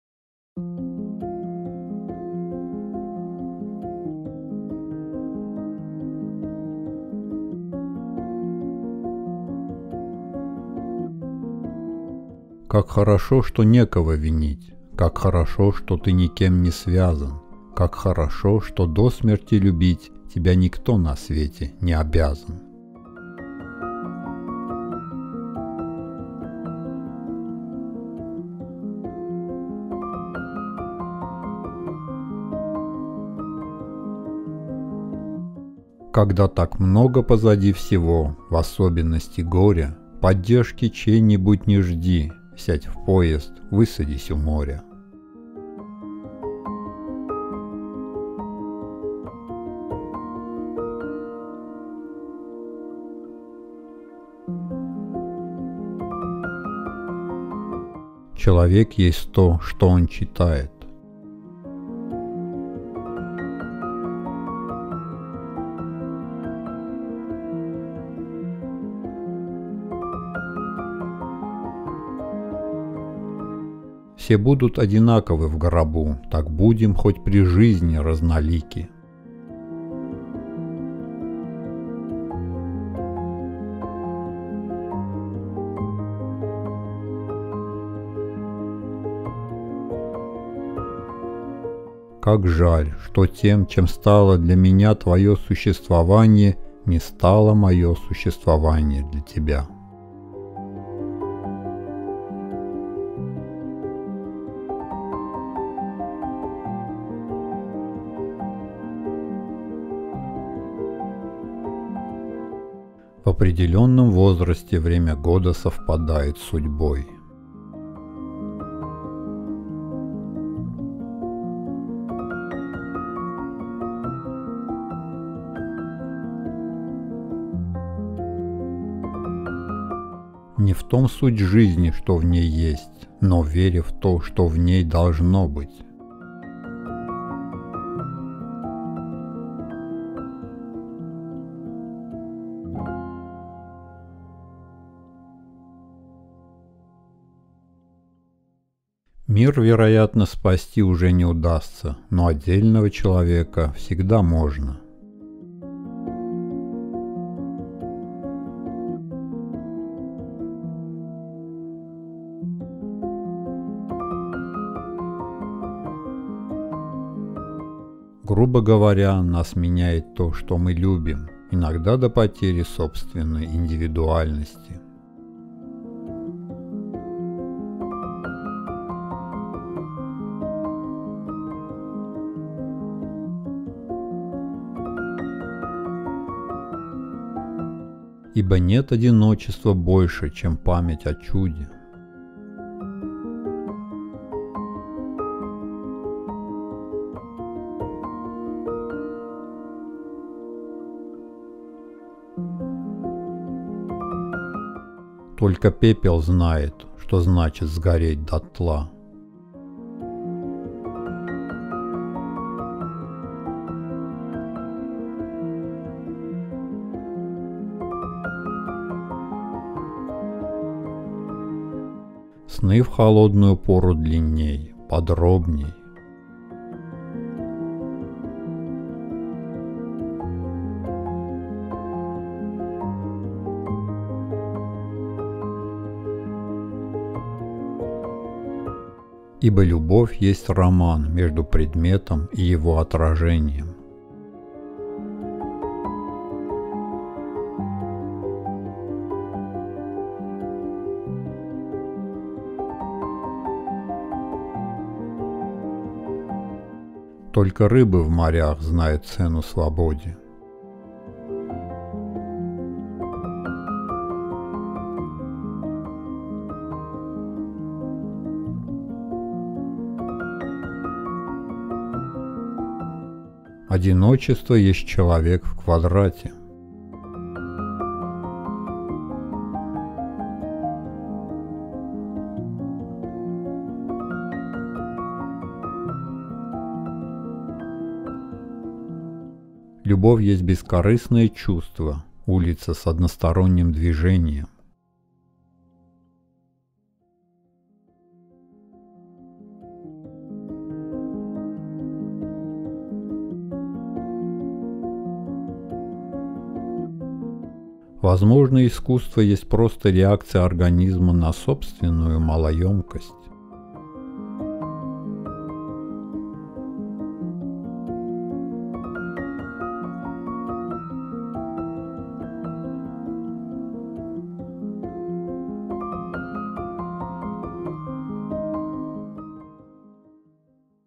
Иосиф Бродский. Только пепел знает, что значит сгореть до тла.
Иосиф Александрович Бродский (24 мая 1940, Ленинград, СССР — 28 января 1996, Нью-Йорк, США) — русский и американский поэт, эссеист, драматург, переводчик, педагог. Лауреат Нобелевской премии по литературе 1987 года, поэт-лауреат США в 1991—1992 годах. Стихи писал преимущественно на русском языке, эссеистику — на английском. Почётный гражданин Санкт-Петербурга (1995).

Цитаты :
Как хорошо, что некого винить, как хорошо, что ты никем не связан, как хорошо, что до смерти любить тебя никто на свете не обязан.
Когда так много позади всего, в особенности - горя, поддержки чьей-нибудь не жди, сядь в поезд, высадись у моря.
... Человек есть то, что он читает...
Все будут одинаковы в гробу.
Так будем хоть при жизни разнолики!
Как жаль, что тем, чем стало для меня твое существование, не стало мое существованье для тебя.
В определенном возрасте время года совпадает с судьбой.
Не в том суть жизни, что в ней есть, но в вере в то, что в ней должно быть.
Мир, вероятно, спасти уже не удастся, но отдельного человека всегда можно.
Грубо говоря, нас меняет то, что мы любим, иногда до потери собственной индивидуальности.
Ибо нет одиночества больше, чем память о чуде.
Только пепел знает, что значит сгореть до тла.
Сны в холодную пору длинней, подробней.
Ибо любовь есть роман между предметом и его отражением.
Только рыбы в морях знают цену свободе
Одиночество есть человек в квадрате.
Любовь есть бескорыстное чувство, улица с односторонним движением.
Возможно, искусство есть просто реакция организма на собственную малоемкость.